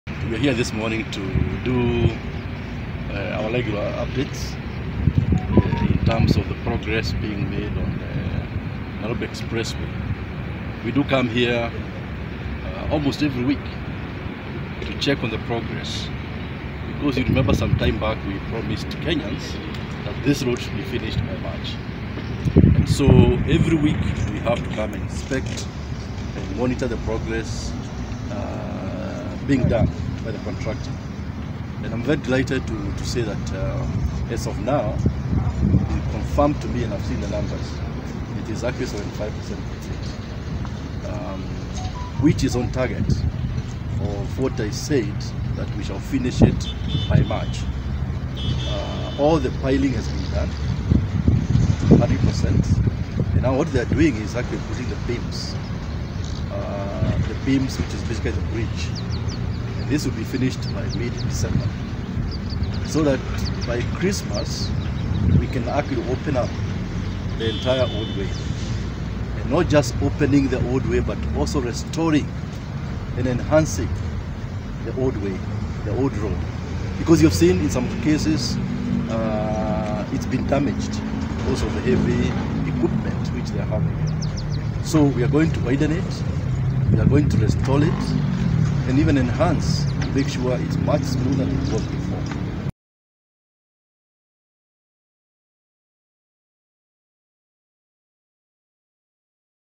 CS Macharia: Nairobi Expressway 75% complete, old roads to be reopened by Christmas
City motorists can breathe a sigh of relief after the Government announced it will reopen the old Waiyaki-Uhuru Highway -Mombasa road affected by the Nairobi Expressway by December 25, 2021.